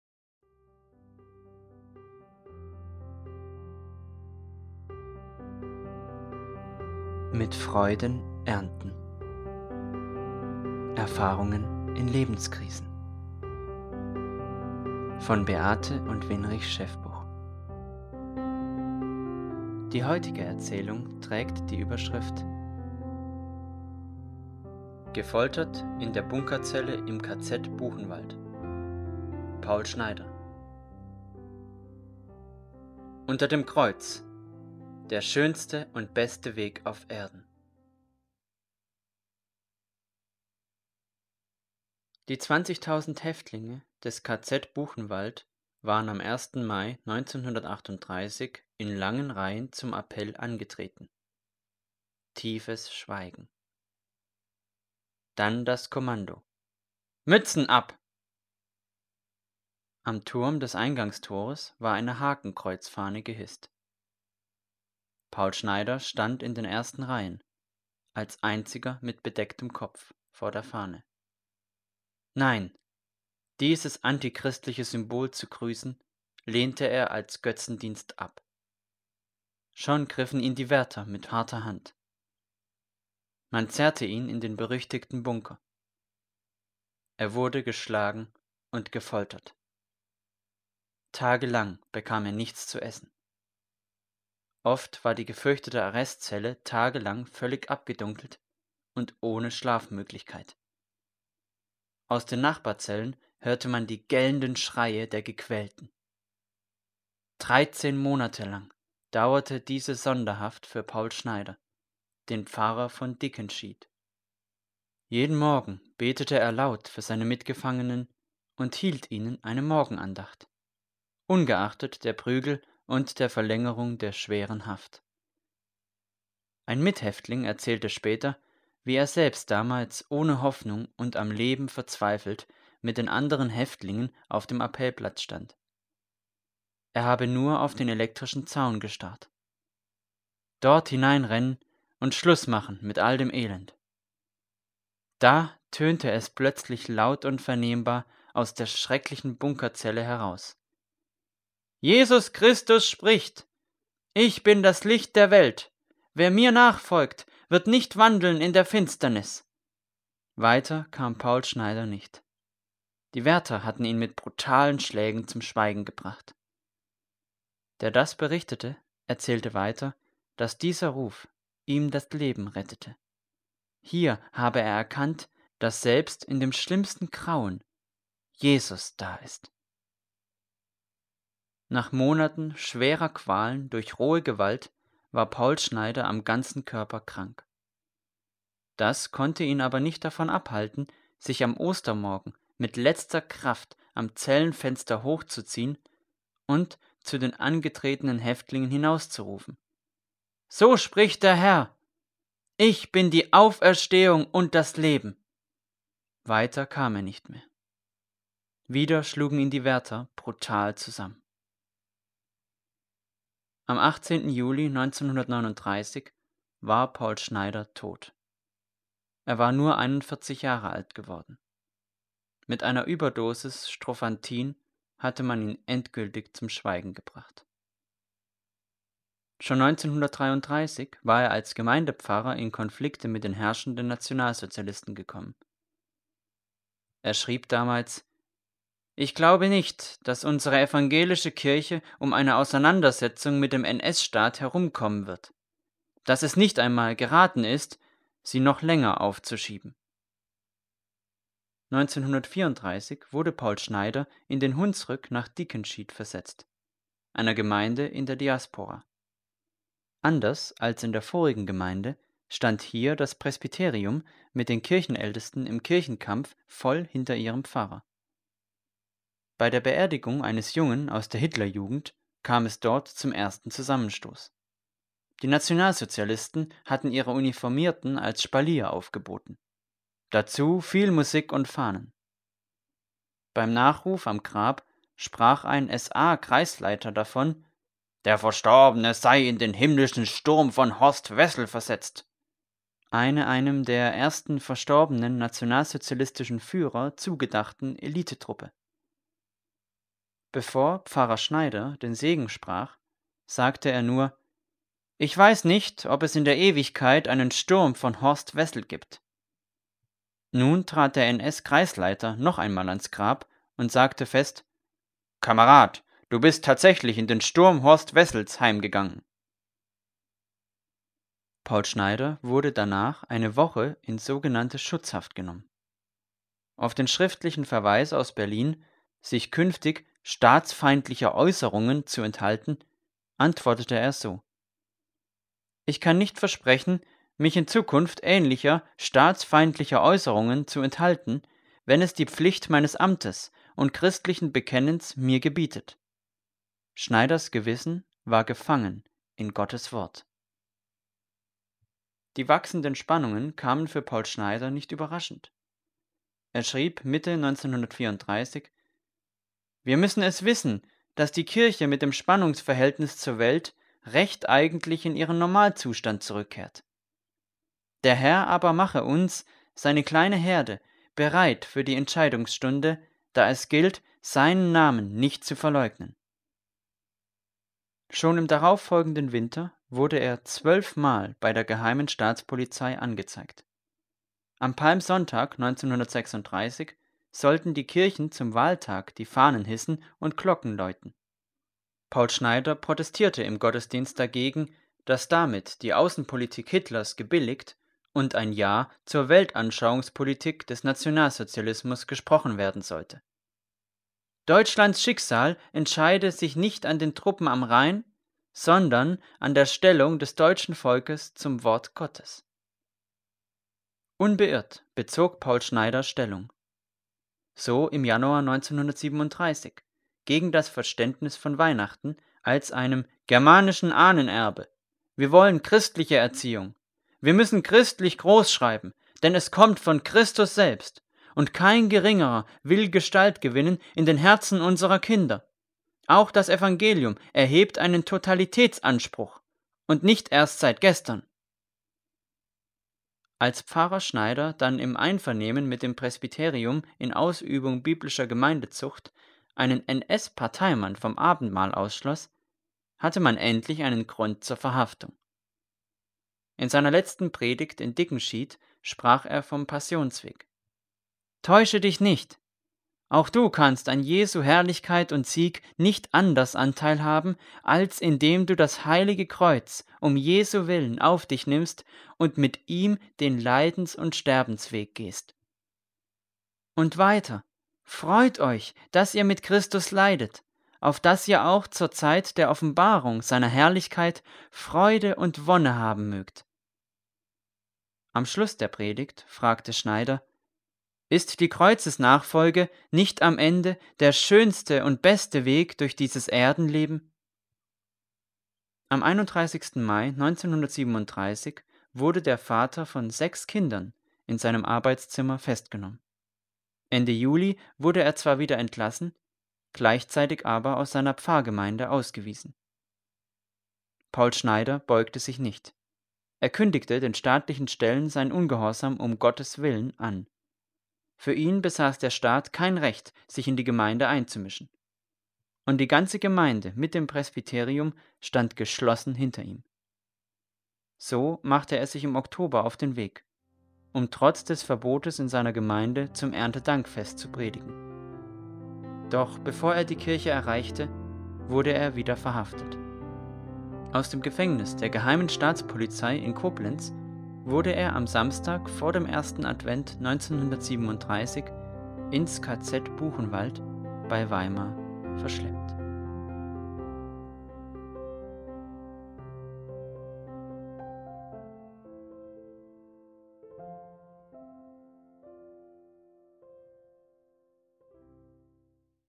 Gefoltert in der Bunkerzelle im KZ Buchenwald - Paul Schneider - Mit Freuden Ernten - Scheffbuch
Mit Freuden ernten ist ein Buch von Beate und Winrich Scheffbuch über schwere Lebensereignisse einiger Christen.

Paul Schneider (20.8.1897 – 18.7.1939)
Margarete Schneider (Hrsg.): Paul Schneider - Der Prediger von Buchenwald,

Vertonung mit freundlicher Genehmigung vom Autor.